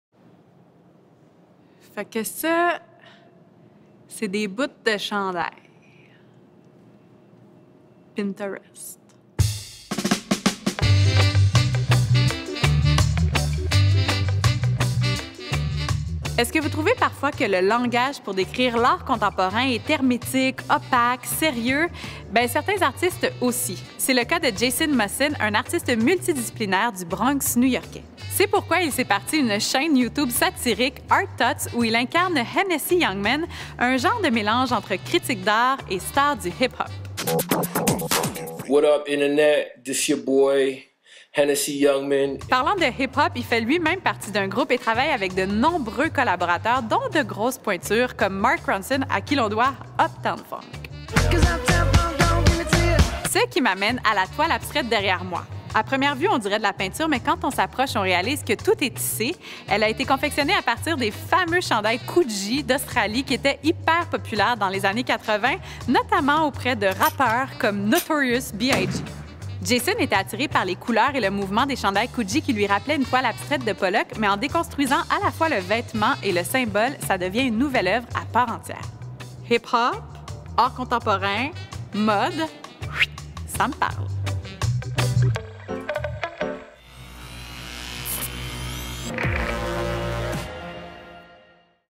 Une oeuvre en (presque) 60 secondes : Jayson Musson, Puerta, 2015
Vanessa Pilon se penche sur l'oeuvre Puerta (2015) de Jayson Musson, dans ce nouvel épisode d'Une oeuvre en (presque) 60 secondes.

Crédits
Animation - Vanessa Pilon
Réalisation - Vanessa Cournoyer
Direction photo et montage - Mathieu-David Crépin
Recherche - Camille L. Prairie et Joséphine Rivard
Prise de son - Marc P Desaulniers
Mix son - François Del Fante
Maquillage - Jennifer Dionne
Assistance caméra - Alexandre Duval